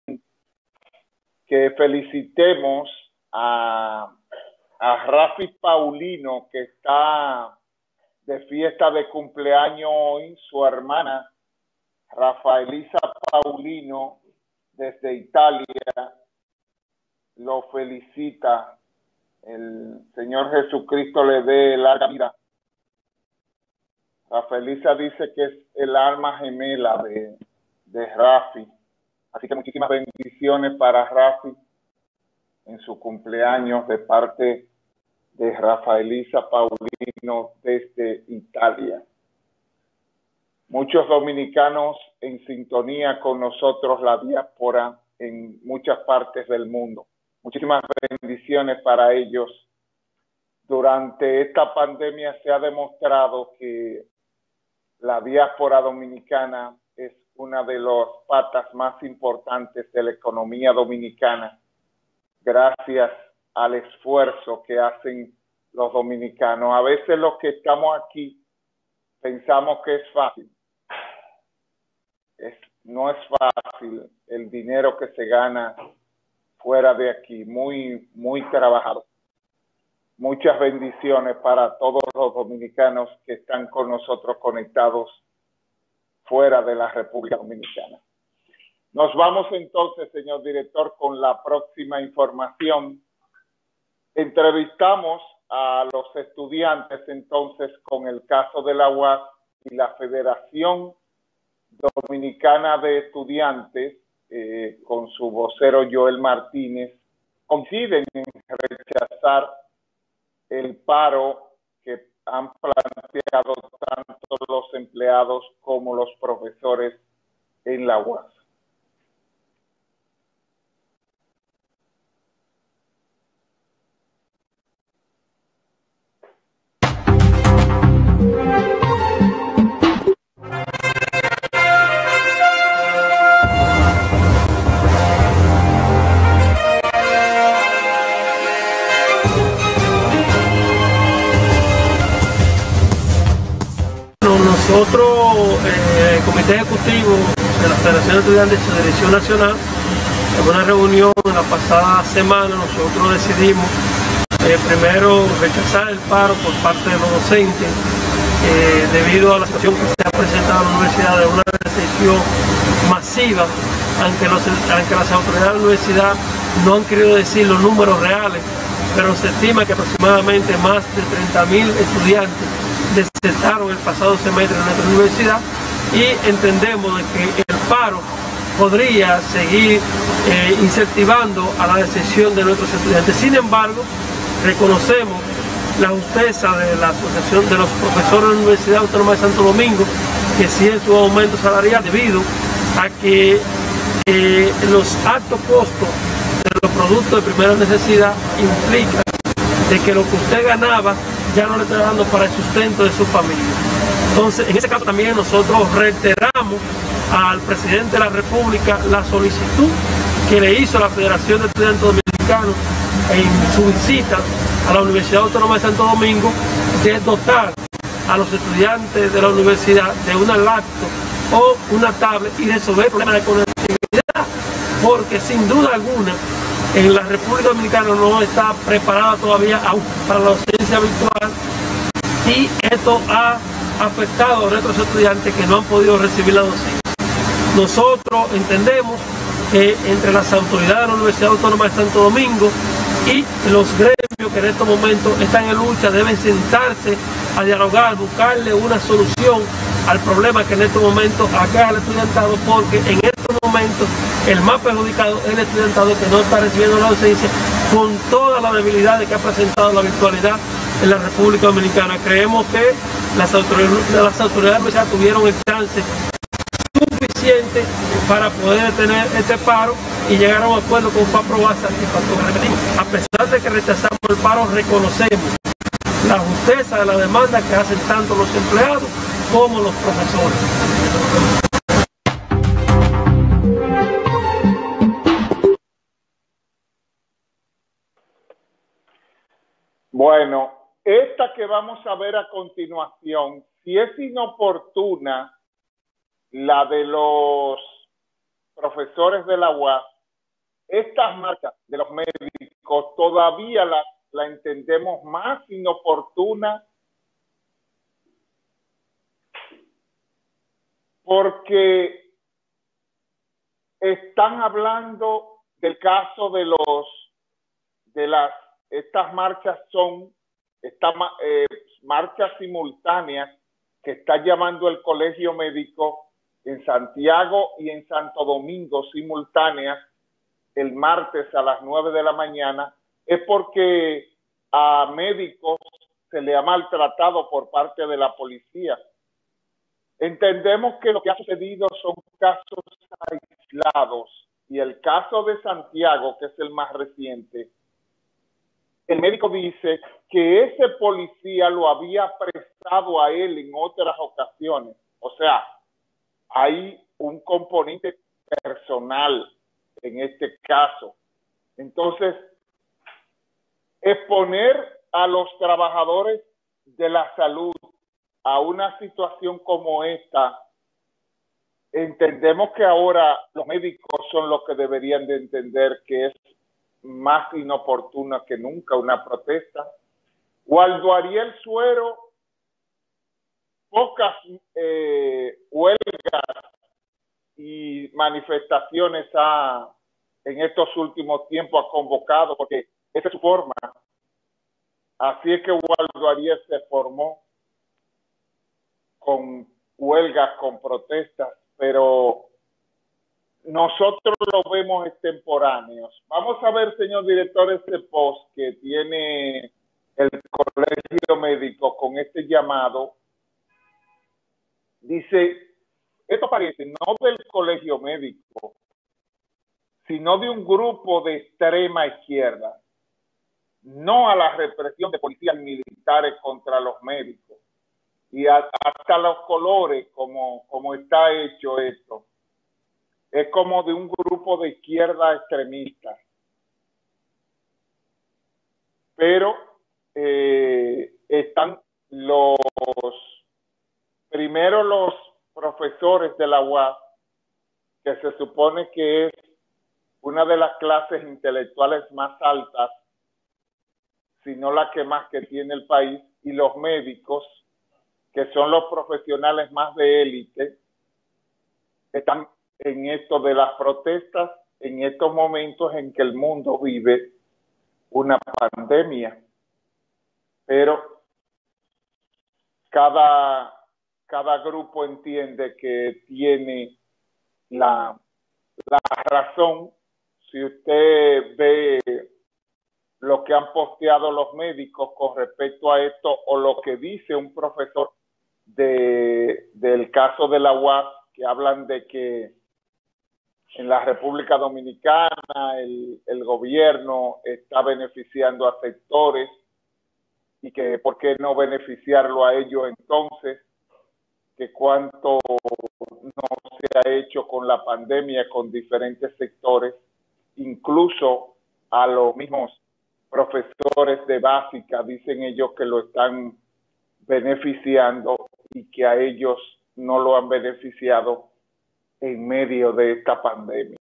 Protesta de los médicos por atropello de los policías es innecesaria e inoportuna
Protesta de los médicos por atropello de los policías es innecesaria e inoportuna según José Manuel Frías